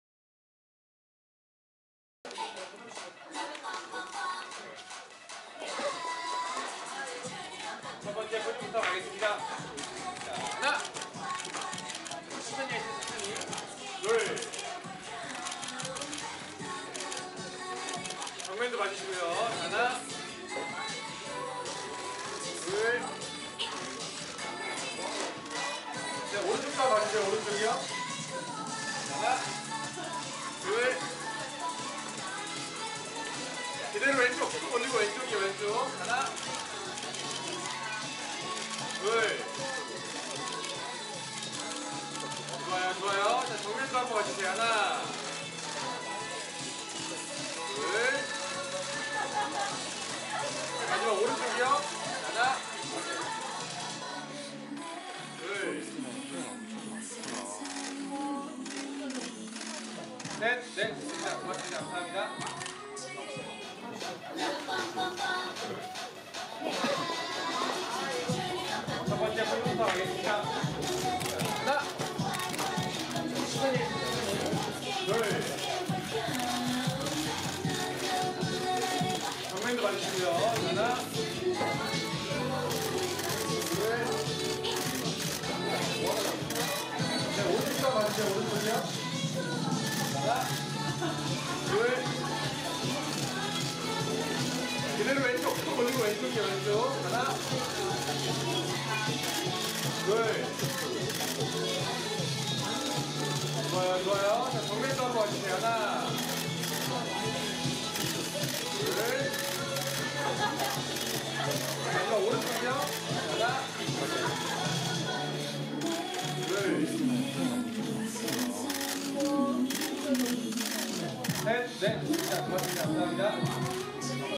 181120 '프로미스나인(fromis_9)' 박지원 팩트인스타 개별 포토타임 세로X가로 2화면 직캠
20일 오후 서울 마포구 상암동 tbs 1층 오픈스튜디오에서 진행된 tbs '팩트인스타 팩터뷰' 공개녹화 현장에서 프로미스나인(fromis_9) 박지원이 포즈를 취하고 있다. 세로, 가로 2개 영상 직캠